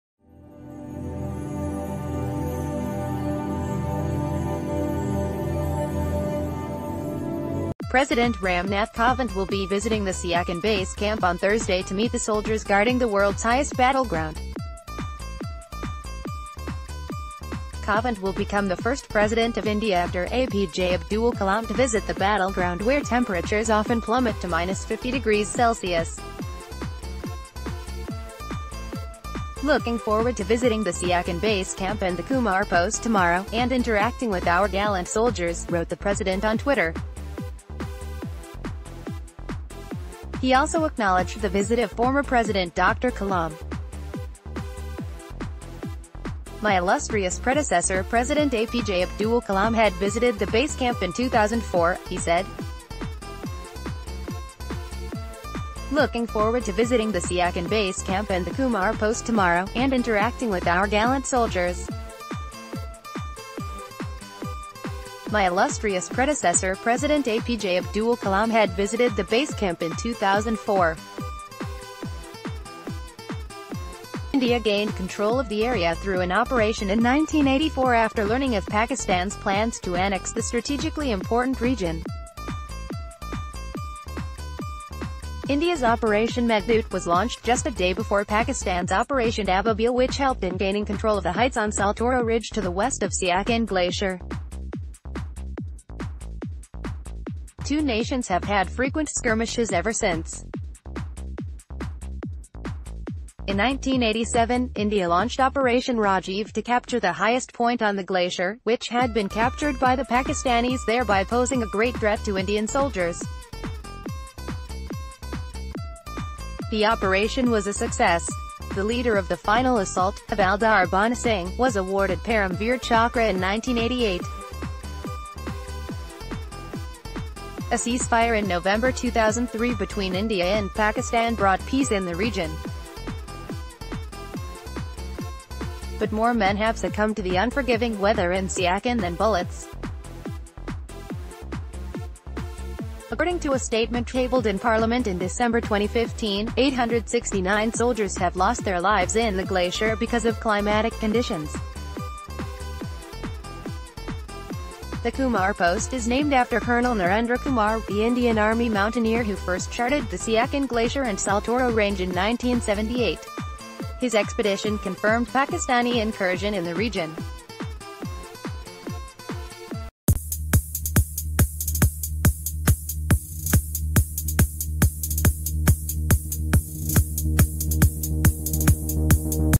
Breaking News | President Ram Nath Kovind to visit Siachen today
President Ram Nath Kovind will be visiting the Siachen base camp on Thursday to meet the soldiers guarding the world’s highest battleground. Kovind will become the first President of India after APJ Abdul Kalam to visit the battleground where temperatures often plummet to -50 degrees Celsius. “Looking forward to visiting the Siachen Base Camp and the Kumar Post tomorrow, and interacting with our gallant soldiers,” wrote the President on Twitter. He also acknowledged the visit of former president...